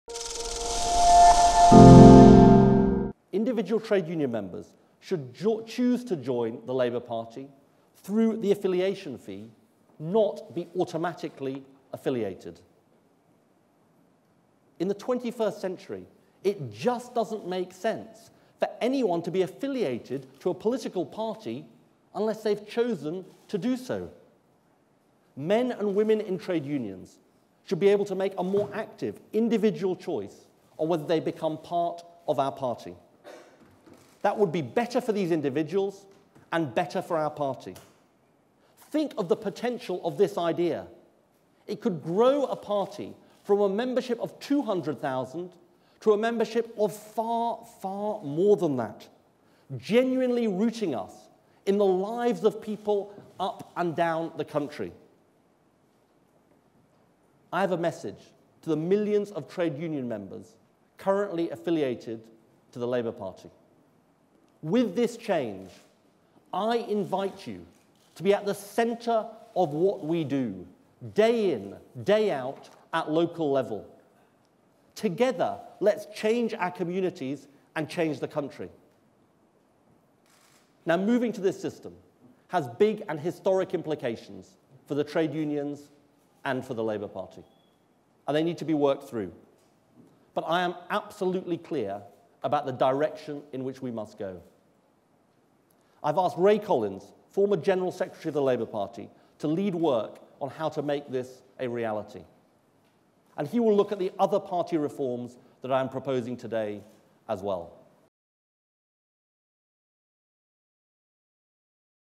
Ed Miliband to reform Labour's historic links to unions
Miliband says that union members should make a 'deliberate' choice to support the party rather than joining automatically. The Labour leader believes that a new opt-in system would increase party membership.